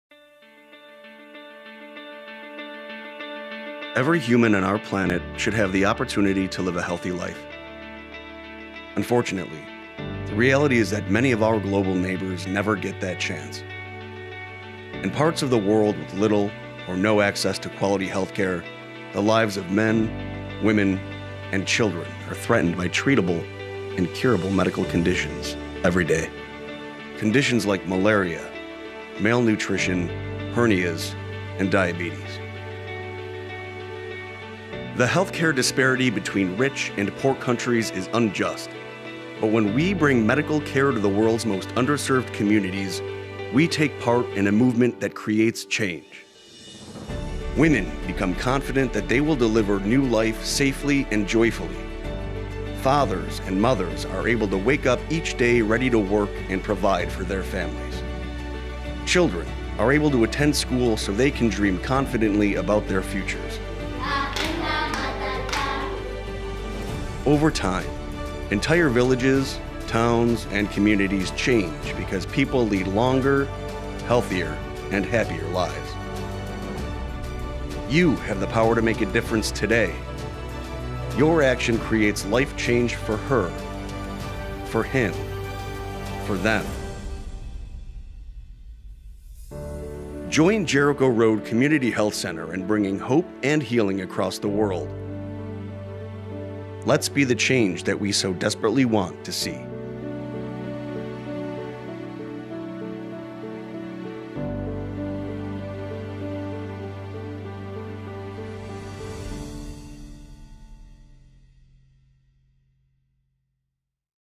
Jericho Road Across the World
Every human on our planet should have the opportunity to live a healthy life. Unfortunately, the reality is that many of our global neighbors never get that chance. The healthcare disparity between rich and poor countries is unjust - but when we bring medical care to the world's most underserved communities, we take part in a movement that creates change. Join us!